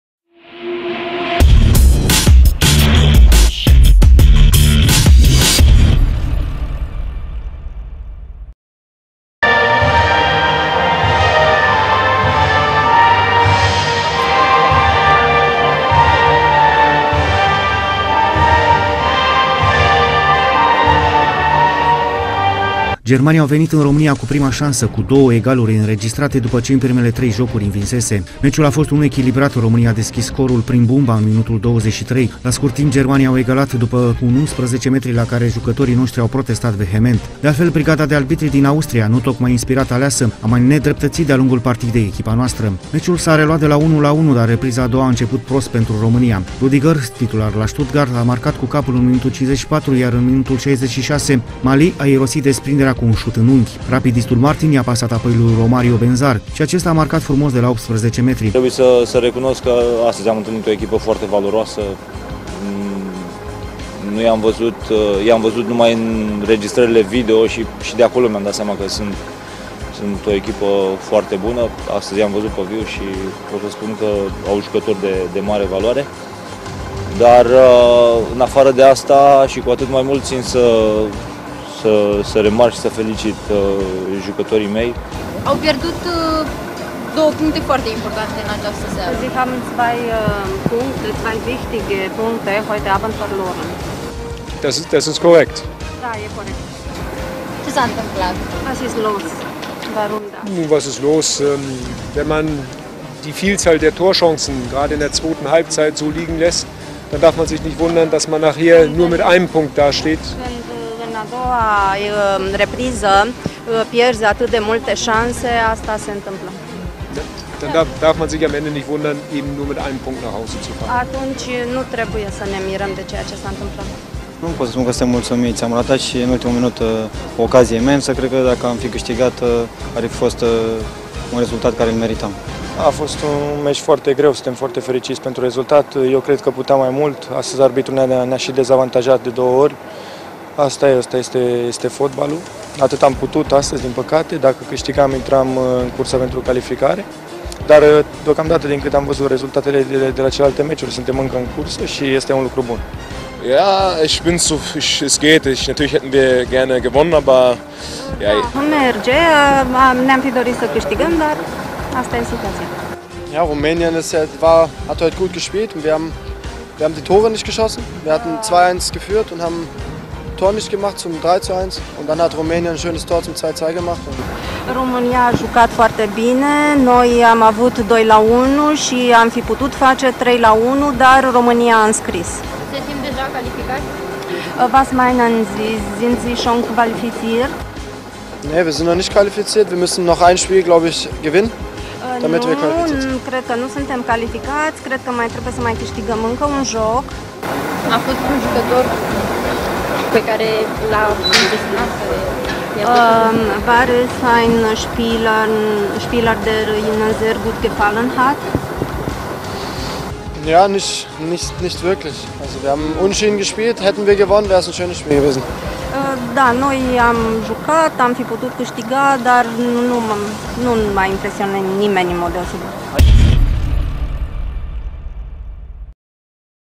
ROMANIA-GERMANIA, LA GIURGIU
Aproximativ 2.500 de spectatori au urmărit meciul dintre echipele de tineret ale României și Germaniei. Meciul s-a disputat la Giurgiu, pe stadionul Marin Anastasovici. A fost o partidă cu un rezultat mult mai bun decât România-Grecia.